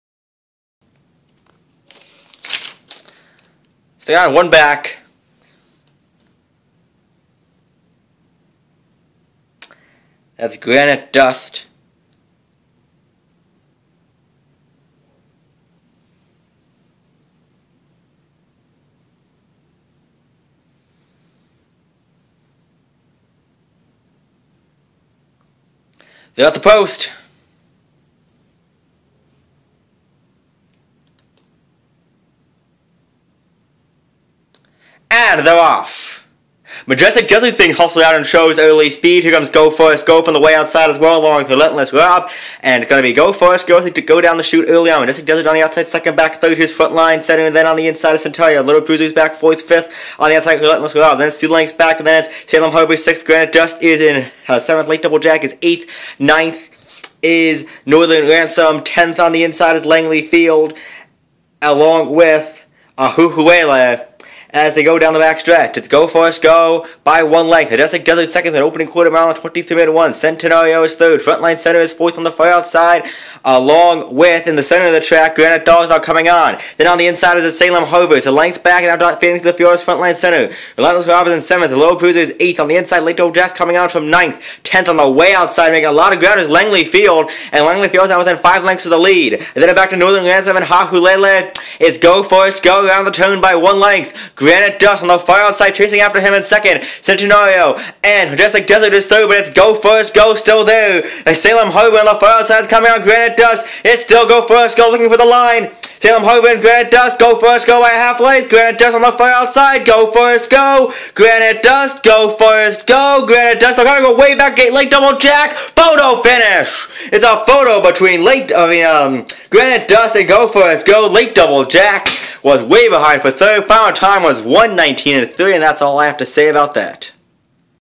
Race 3 at Presque Isle Downs 8/27/13
Full field and a close finish at Presque Isle Downs.